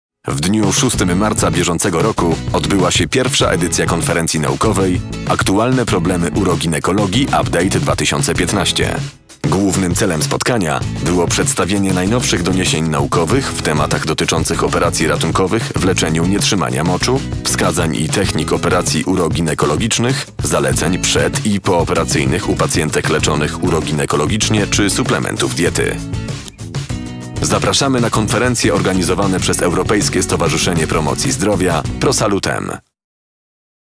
I Konferencja Naukowa „Aktualne problemy Uroginekologii – UPDATE-2015" - podsumowanie
Podsumowanie I Konferencji Naukowej „Aktualne problemy Uroginekologii – UPDATE-2015", która odbyła się 6 marca 2015 r. w Warszawie